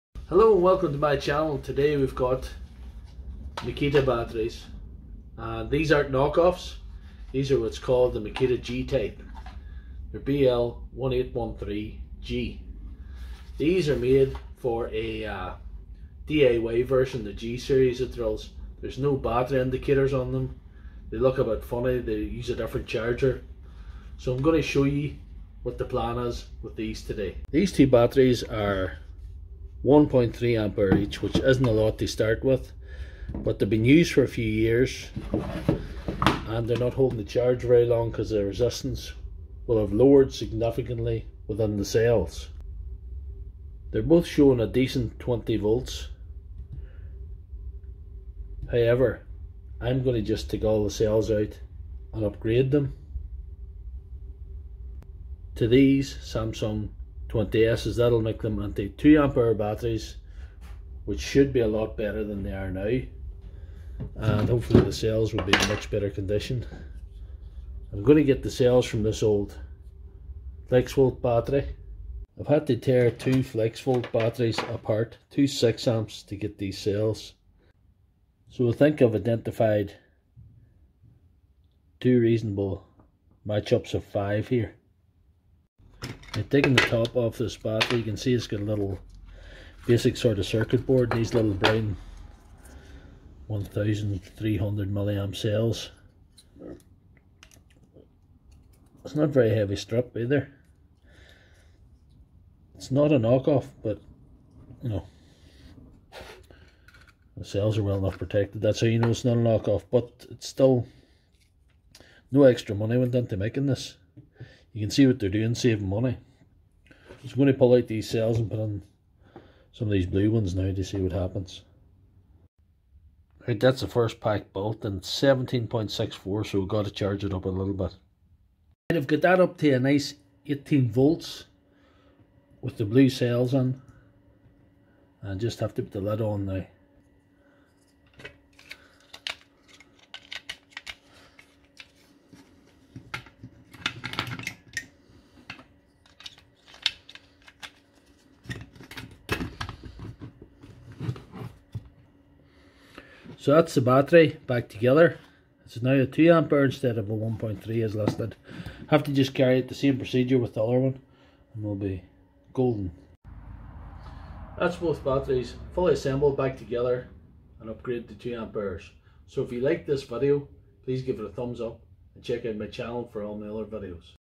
Giving Makita G Type Batteries more power
I upgraded these worn out underpowered makita grinder type 18v batteries using Samsung cells 2000mah from flexvolt 6ah 54v batteries. This would nearly double the runtime and help overall power. Battery repair is a risky undertaking and careful consideration should be taken before attempting it. It is not the intention of Doctor Lefthandthread media for any injury to occur as a result of my videos.